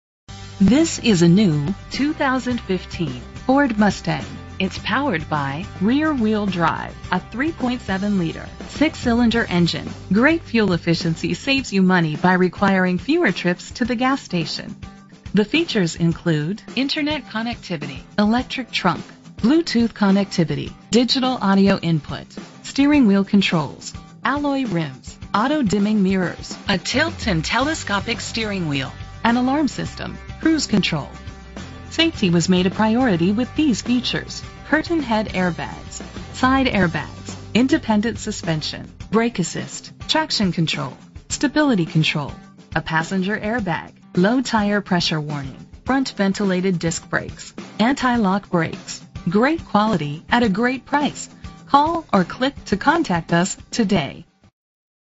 2015 Ford Mustang - Oklahoma City OK
Phone:
Year: 2015
Make: Ford
Model: Mustang
Trim: V6
Engine: 3.7 liter 6 cylinder 24 valve
Transmission: 6-Speed
Color: Oxford White
Mileage: 6
Address:
7320 NW Expressway